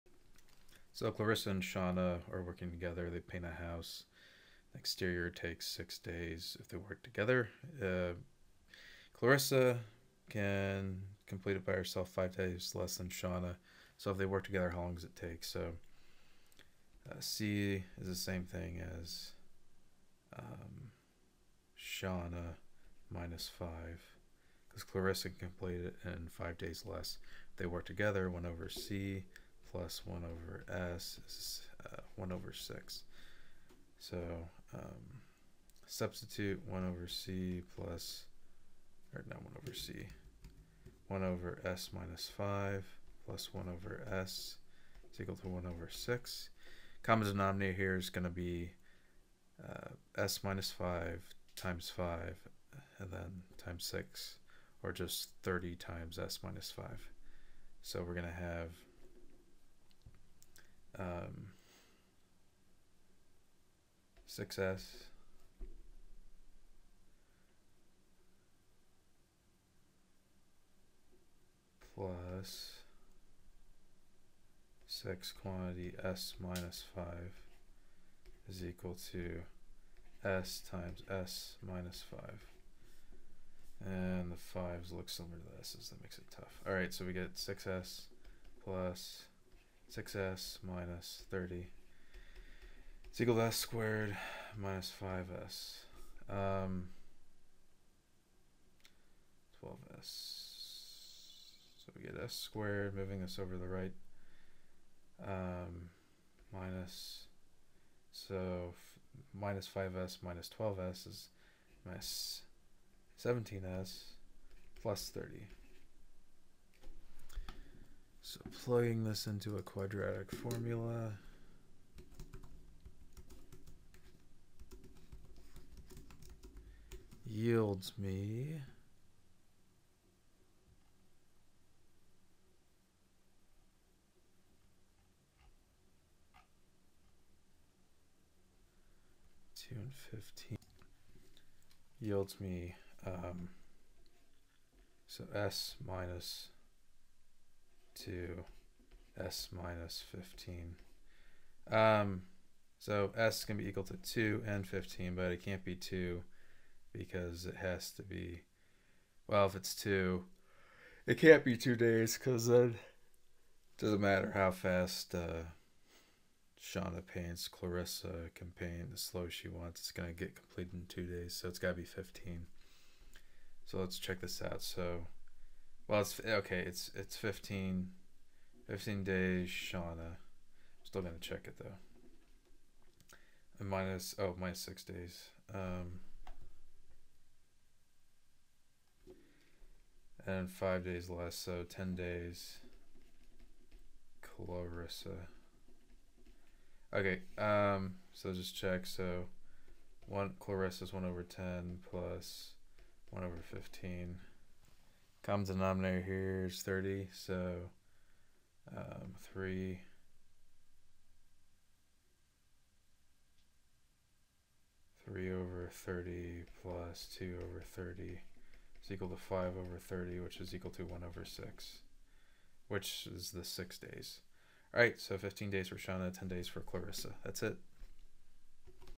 Clarissa and Shawna, working together, can paint the exterior of a house
Working Together to Get a Job Done Clarissa and Shawna, working together, can paint the exterior of a house in 6 days. Clarissa by herself can complete this job in 5 days less than Shawna.HowlongwillittakeClarissatocompletethejobby herself?